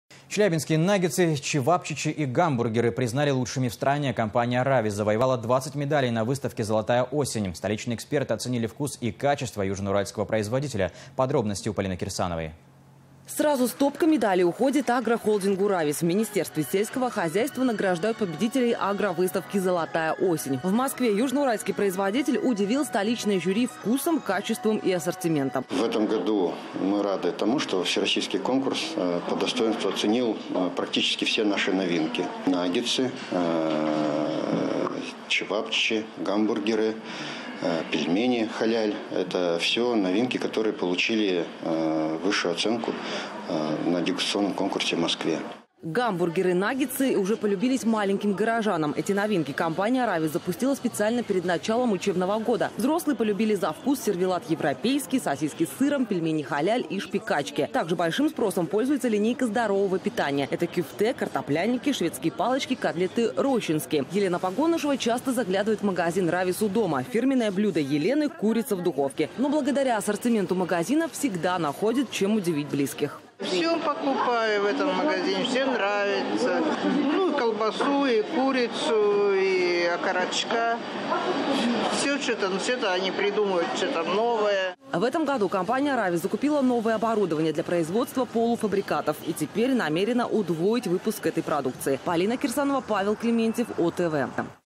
Челябинские наггетсы стали лучшими в стране
Челябинские нагетсы, чивапчичи и гамбургеры признали лучшими в стране. Компания Равис завоевала 20 медалей на выставке «Золотая осень». Столичные эксперты оценили вкус и качество южноуральского производителя. Подробности — Полины Кирсановой.